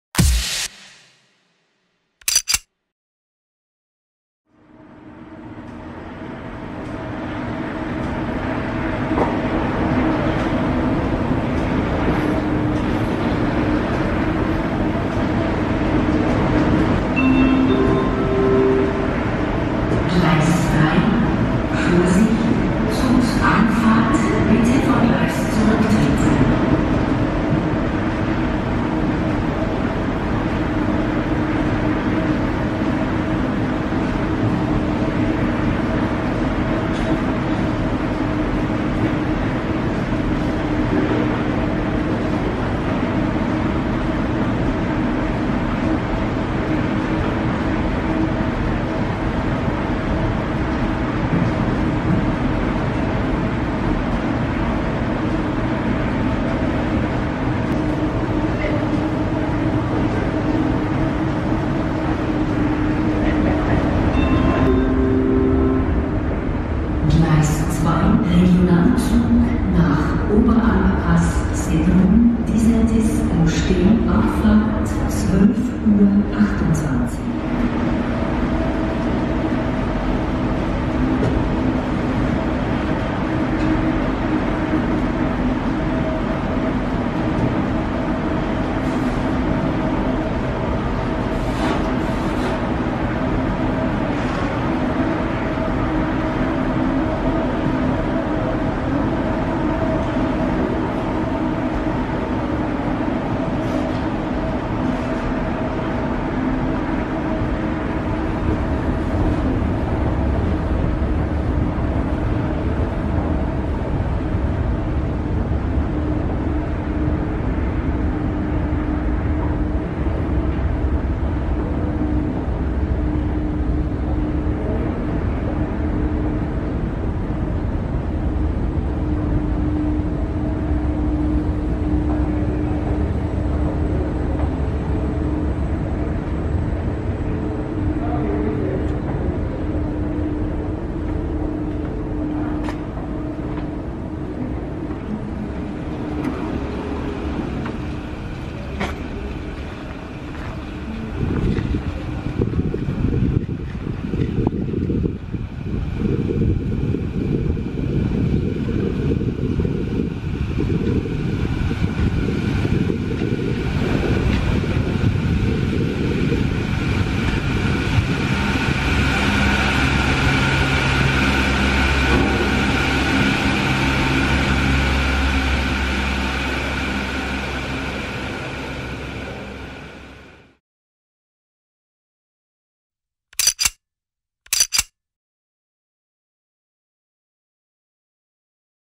New Station Andermatt | World Wide
From the new underpass on the east side of the Matterhorn Gotthard Bahn station Andermatt to the Gütsch-Express ropeway. Because you can not walk well with ski boots on, 'Swiss Alps' spends a conveyor belt. But only one way - to the ski area. For now, it is possible to come by cable cars and ski slopes from Andermatt almost to Sedrun, to Dieni. If everything works. Switzerland, Dec 18, 2018.